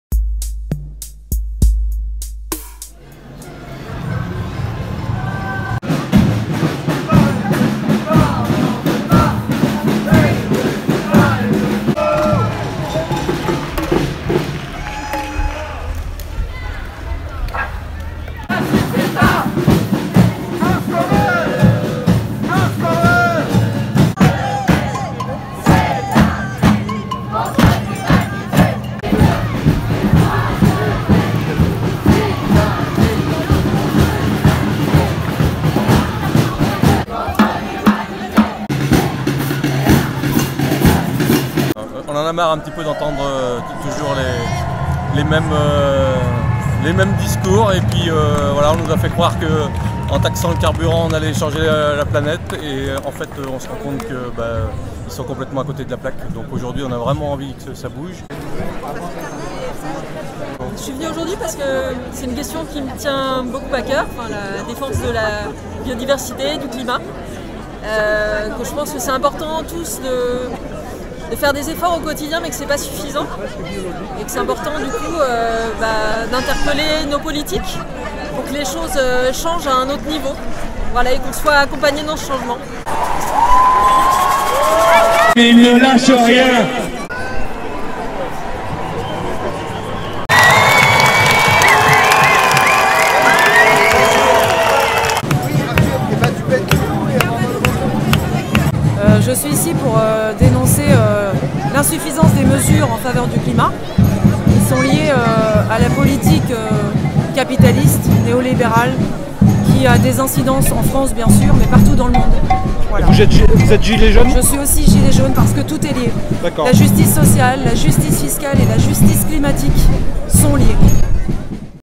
VIDÉO.Poitiers : 2000 personnes pour le climat
La Marche du siècle a rassemblé à Poitiers autant que le rassemblement des jeunes et lycéens la veille. La cause du climat mobilise les Poitevins.